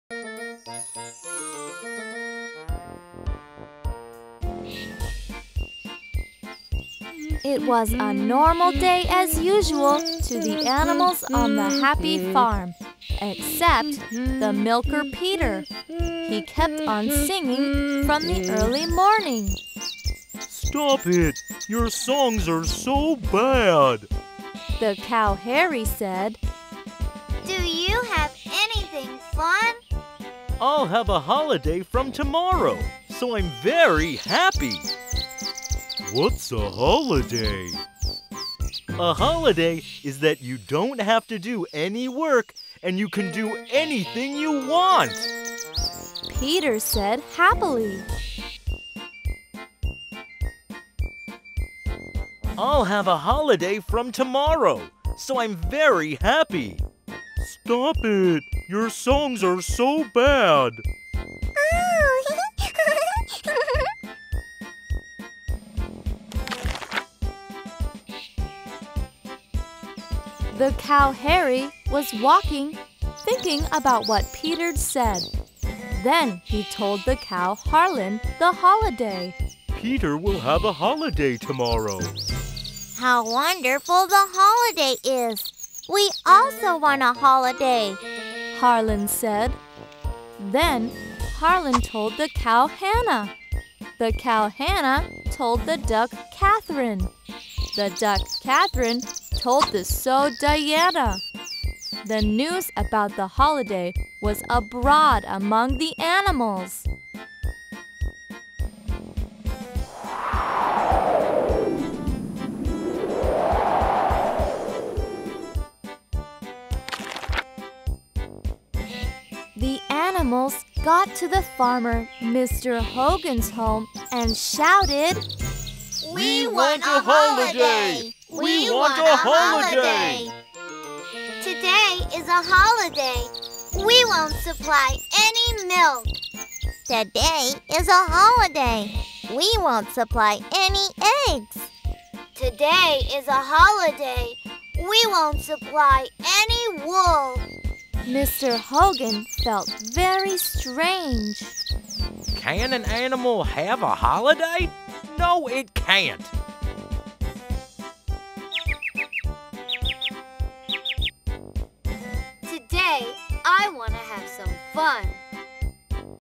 Book Apps | Today Is a Holiday - 3D Interactive Pop-up Book
Only The Best Apps Reviewed With Video!
This book app is a pop up full of lively animations and interactive opportunities!  The animals want a holiday & take one! Great opener for dialogue on animals, holidays, and more.In Both English and Chinese!